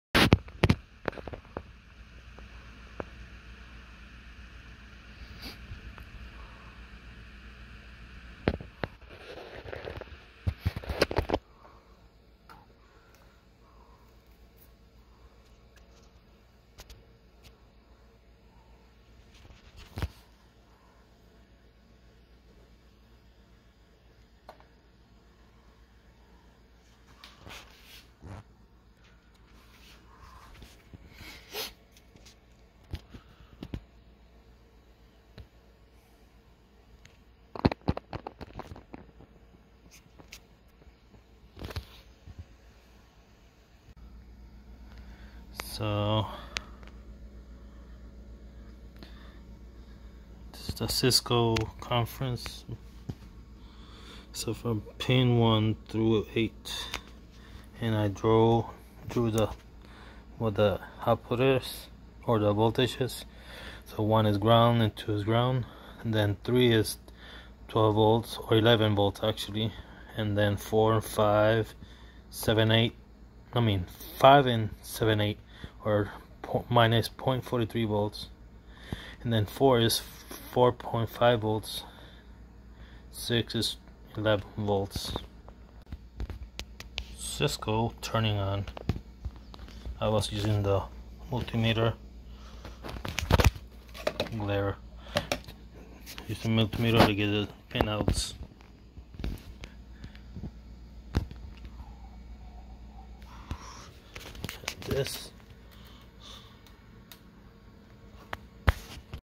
Cisco Conference Display (Part: CTS-CTRL-DV8) Inside the Cisco TelePresence Touch Device.
In this Video I am checking the voltage for the pin for the Cisco TelePresence Touch Device.